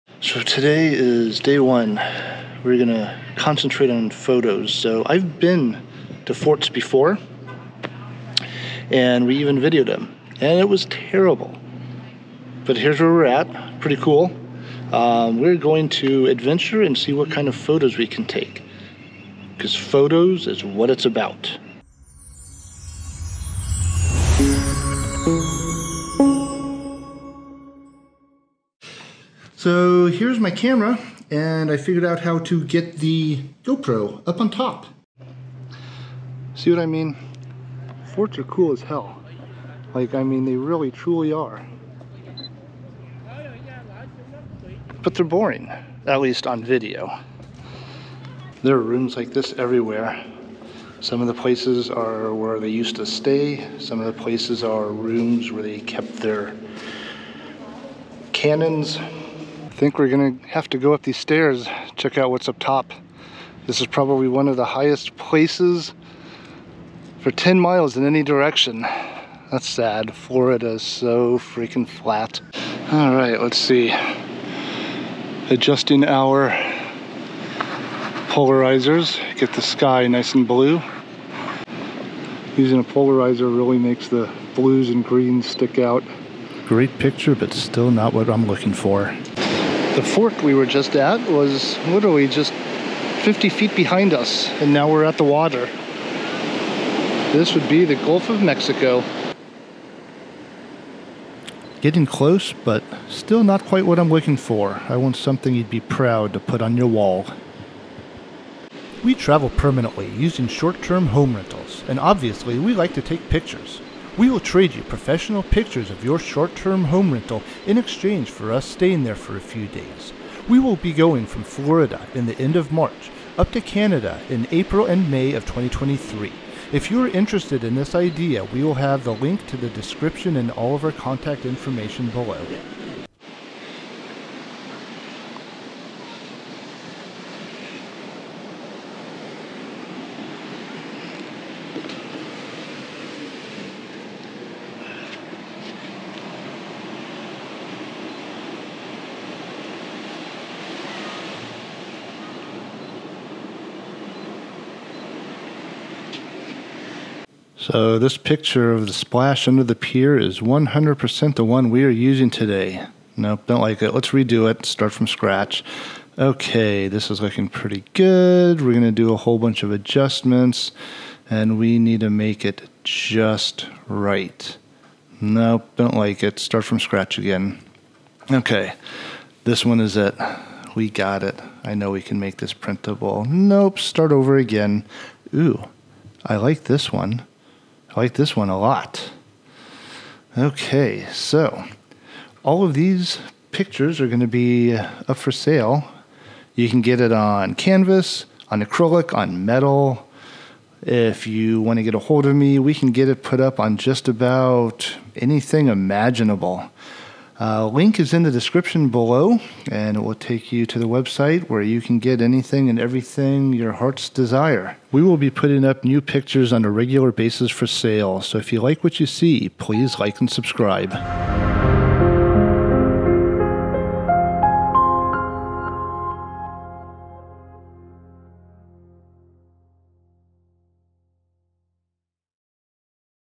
Art Worthy photos while traveling Florida [HD]
We travel full-time. Spring 2023 will be the East Coast from Florida to Canada. Documenting our adventures along the way.  If you like our lifestyle and want to put the picture on your wall here is the link